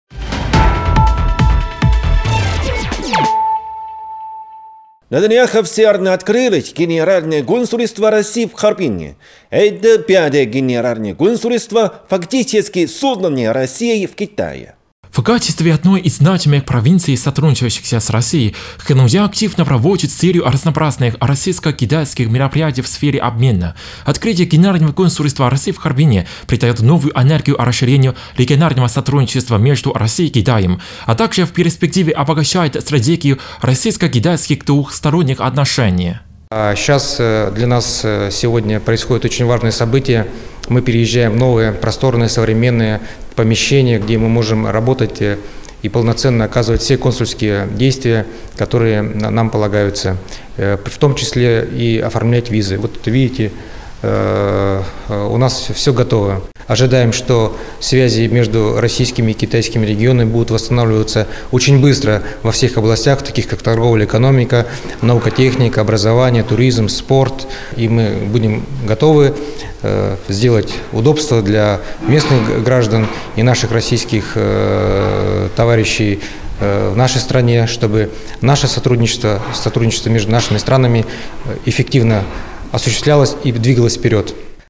Новости провинции Хэйлунцзян. 13.02.2023.
Новый офис Генерального консульства России официально открылось в Харбине.  Главными гостями с китайской стороны стали вице-губернатор провинции Хэйлунцзян Юй Цзянь и директор Консульского департамента МИД КНР У Си.